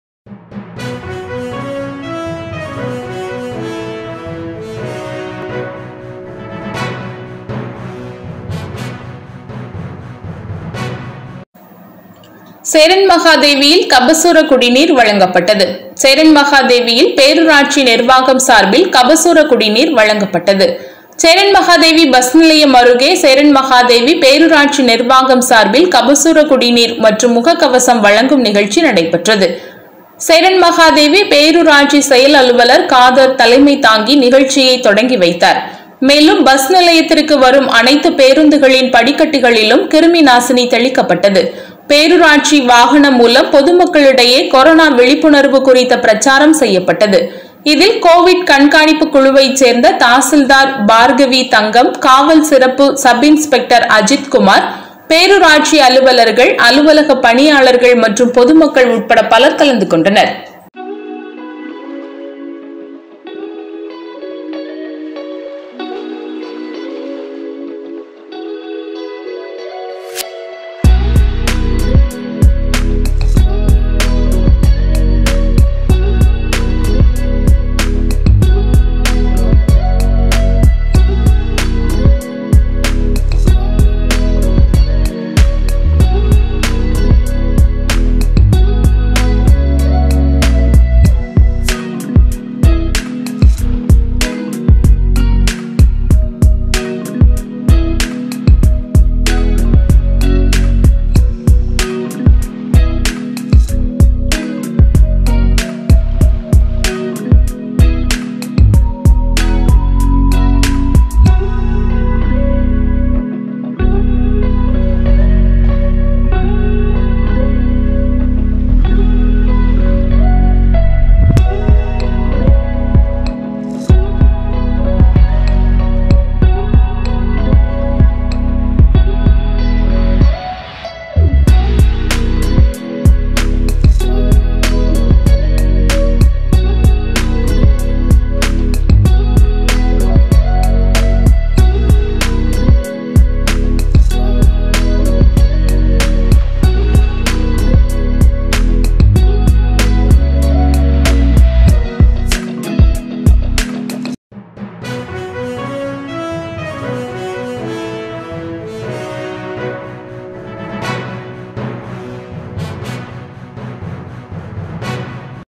சேரன்மகாதேவியில் பேரூராட்சி நிர்வாகம் சார்பில் கபசுர குடிநீர் வழங்கப்பட்டது.
சேரன்மகாதேவியில் கபசுரக் குடிநீர் வழங்கல்.

சேரன்மகாதேவி பஸ் நிலையம் அருகே சேரன்மகாதேவி பேரூராட்சி நிர்வாகம் சார்பில் கபசுர குடிநீர் மற்றும் முகக் கவசம்  வழங்கும் நிகழ்ச்சி நடைபெற்றது. சேரன்மகாதேவி பேரூராட்சி செயல் அலுவலர் காதர் தலைமை தாங்கி நிகழ்ச்சியை தொடங்கி வைத்தார். மேலும் பஸ் நிலையத்திற்கு வரும் அனைத்து பேருந்துகளின் படிக்கட்டுகளிலும் கிருமி நாசினி தெளிக்கப்பட்டது. பேரூராட்சி வாகனம் மூலம் பொதுமக்களிடையே கொரோனா விழிப்புணர்வு குறித்த பிரச்சாரம் செய்யப்பட்டது. இதில் கோவிட் கண்காணிப்புக் குழுவைச் சேர்ந்த தாசில்தார் பார்கவி தங்கம், காவல் சிறப்பு சப்-இன்ஸ்பெக்டர் அஜித்குமார், பேரூராட்சி அலுவலர்கள், அலுவலக பணியாளர்கள் மற்றும் பொதுமக்கள்  உள்பட பலர் கலந்து கொண்டனர்.